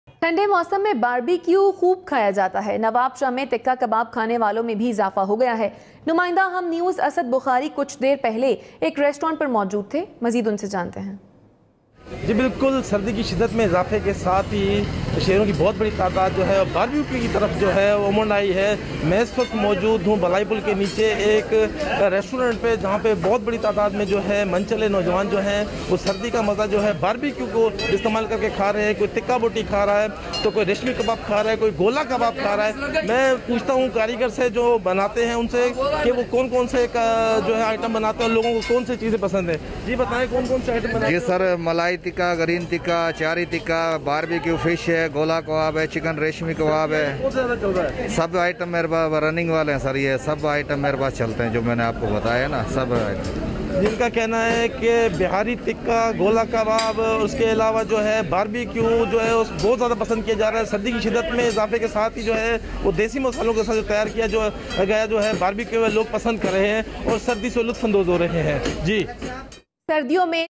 Nawabshah Asad Bukhari Humnews
❤️ہم نیوز سب سے پہلے ❤️
موسم سرد ہوتے ہی نواب شاہ میں بار بی کیو کی مانگ میں اضافہ ہوگیا ہے تکہ, بوٹی, کباب کھانے والے شوقین ہوٹلوں پر پہنچ گئے ہیں اس حوالے سے مزید بتارہے ہیں نمائندہ ہم نیوز اسد بخاری